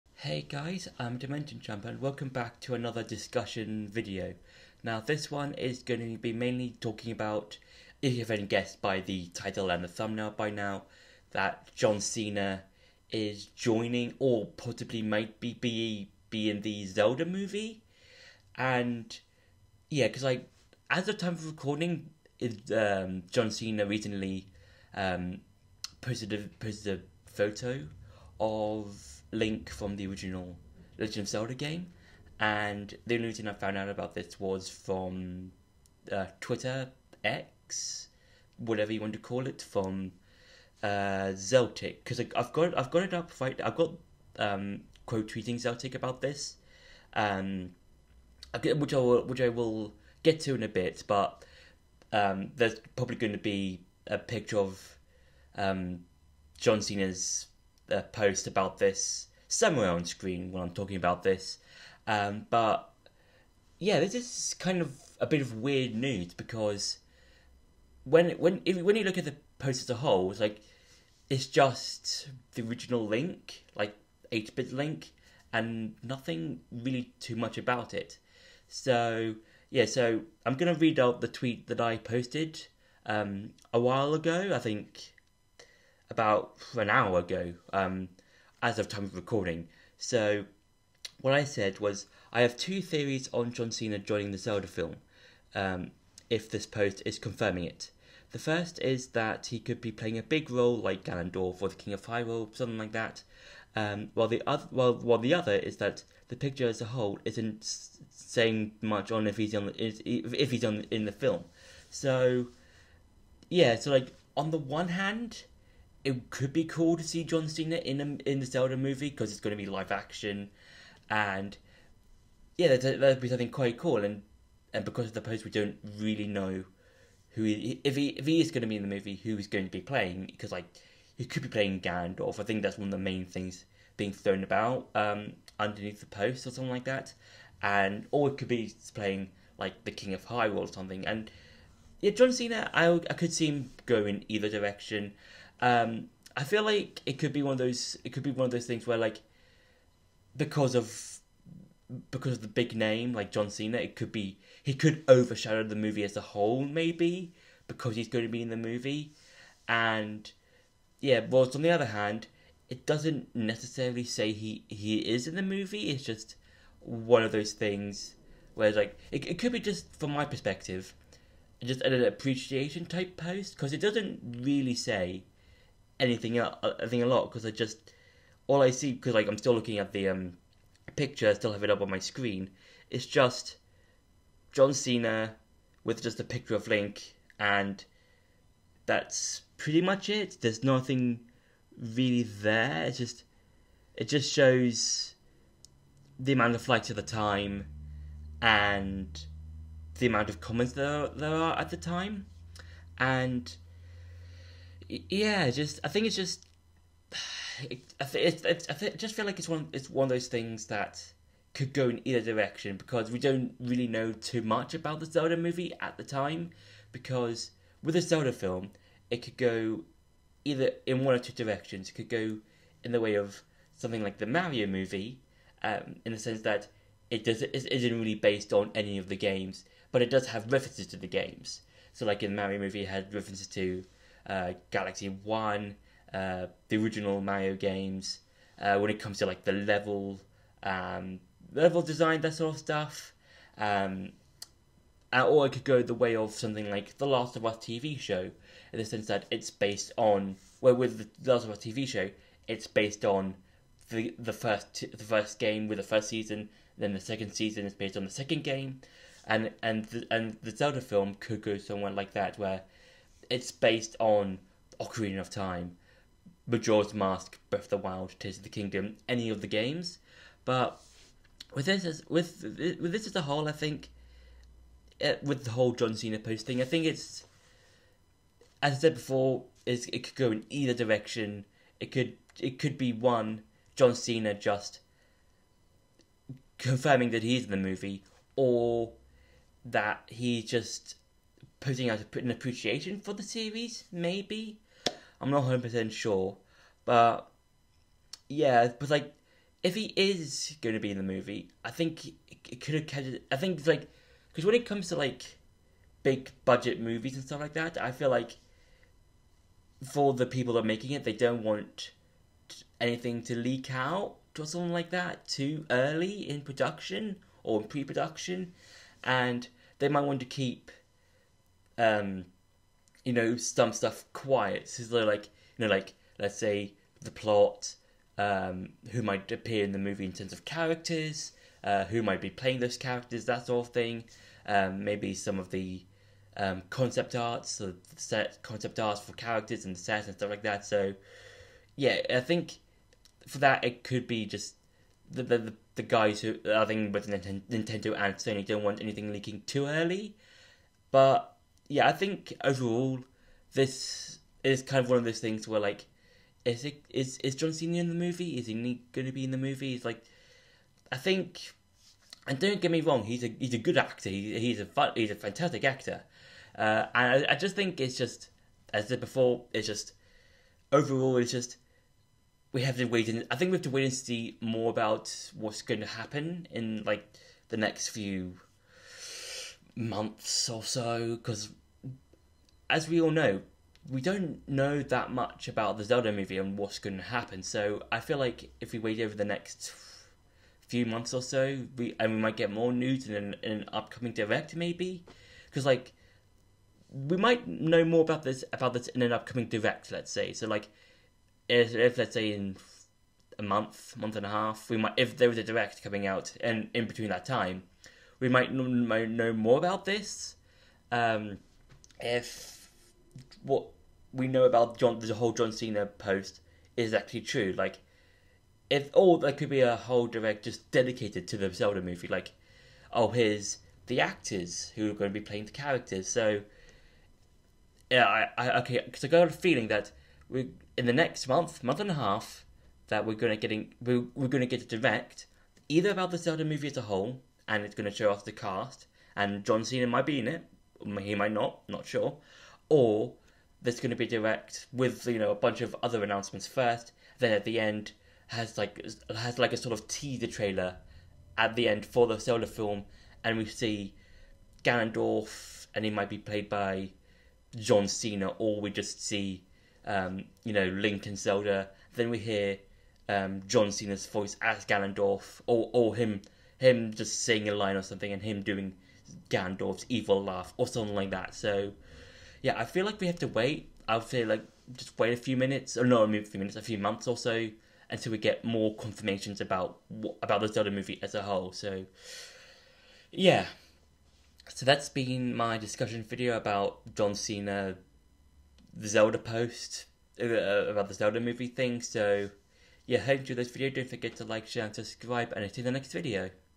Is John Cena joining the Zelda Movie? (Mini General Discussion)
With John Cena's recent post on his Instagram Page of 8-Bit Link, I have quickly thrown together my thoughts on if he is going to be in the Zelda Movie.